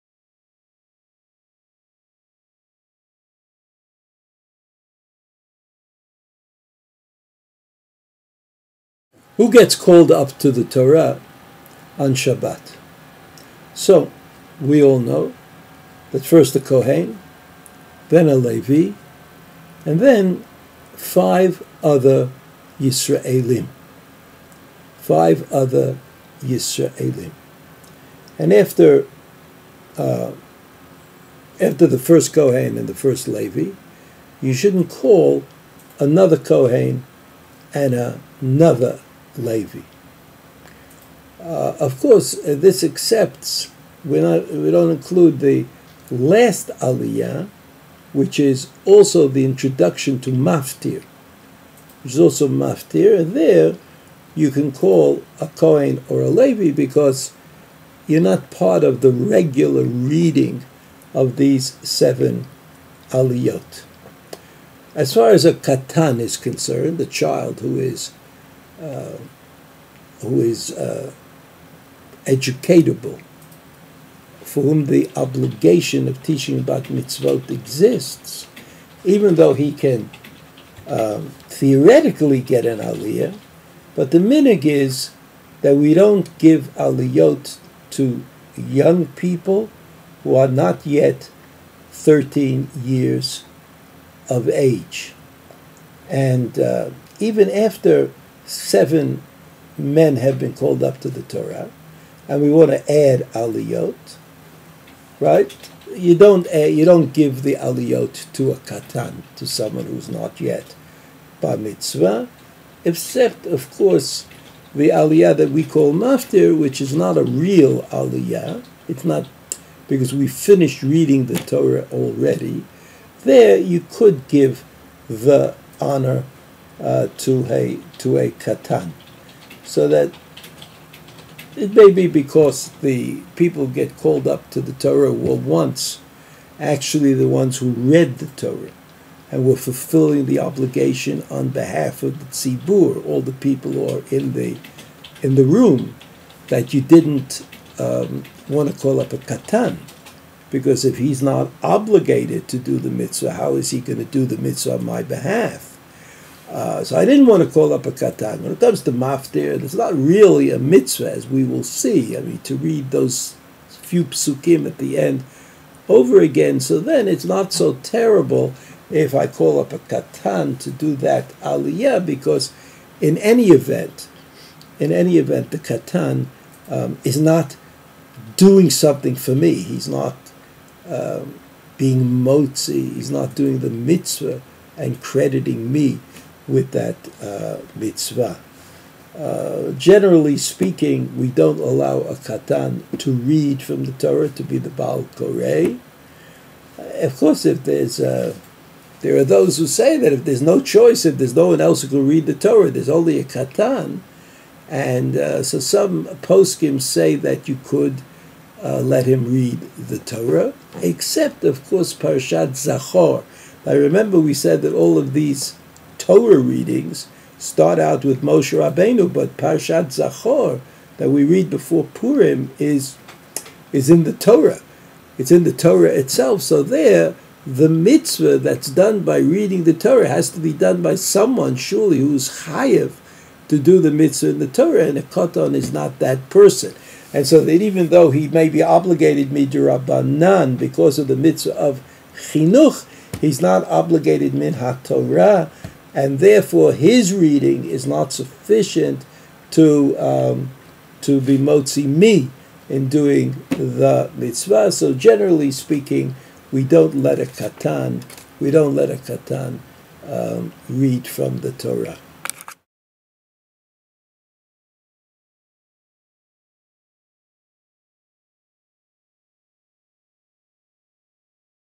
Halacha Yomit: Kriat HaTorah - The Order of Aliyot on Shabbat
To learn more about WebYeshiva or access our full catalog of free and interactive online classes please
In this video, Rabbi Chaim Brovender, of WebYeshiva.org, discusses the order of Aliyot on Shabbat during Kriat HaTorah. First a Kohen, then a Levi and then 5 Yisraelim (Israelites)

WebYeshiva is the world's first fully interactive online Yeshiva and Midrasha.